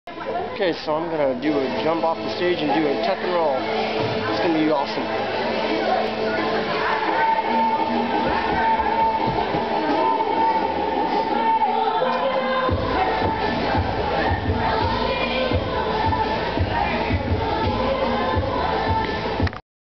Youth group lock-in
Video #86. Taken: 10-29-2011. This is what you get when I stay up all night and take a video at 6:23 AM. By the way, I was up at 7 AM the previous morning. So I had almost been up a complete 24 hours.